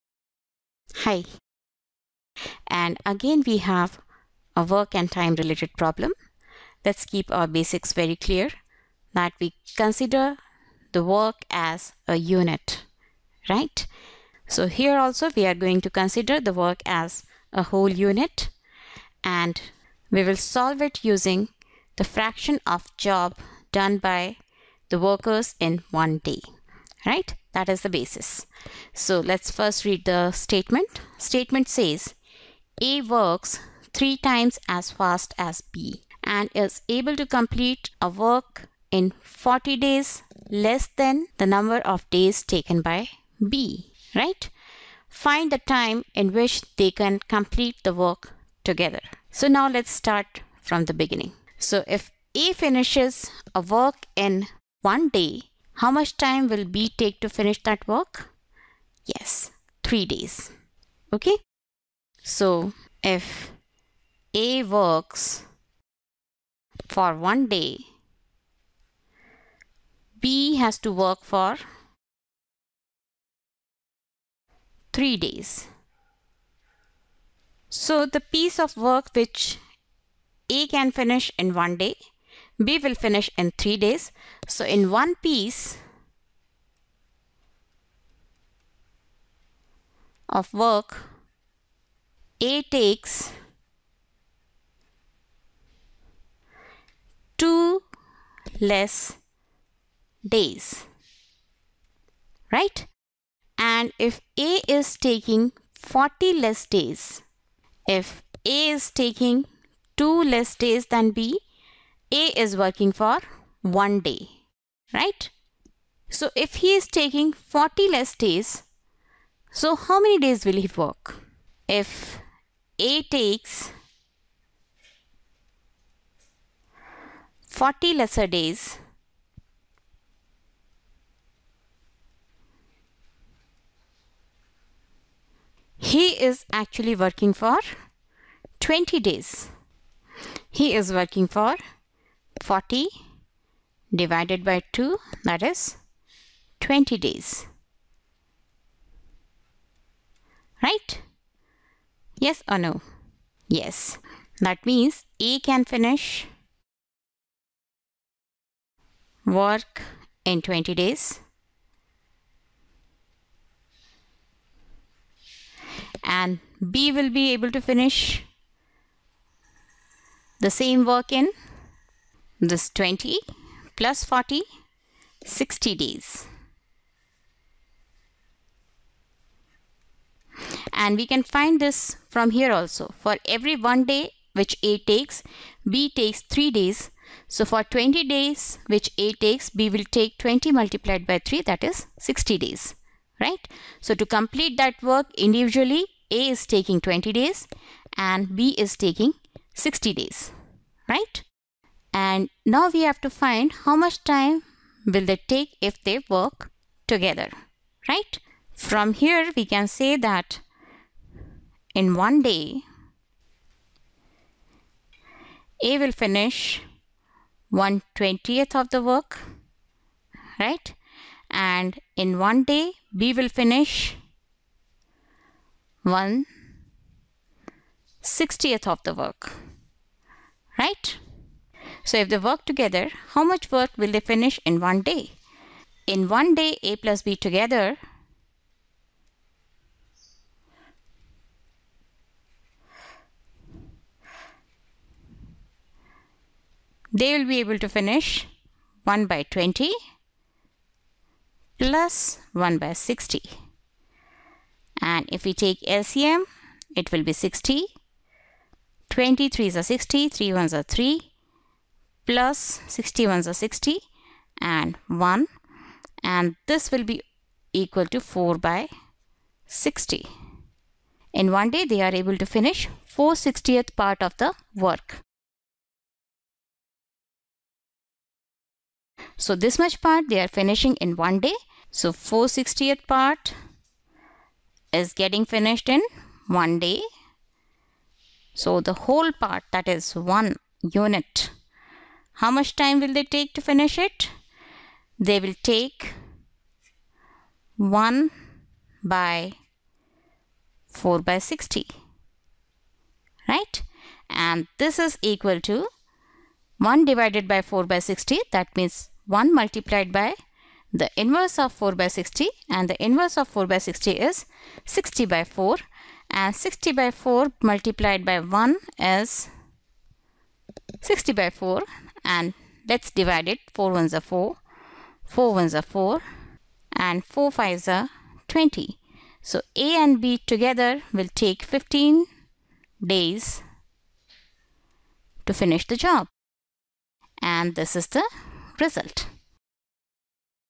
Work time word problem with two speeds
A works 3 times as fast as B and completed work in 40 days less than time taken by B. find time taken if they work together on the same task.

General Mental Ability Problem for IAS, UPPSC, MPPSC, TNPSC, CAT and other aptitude tests.  For more such videos.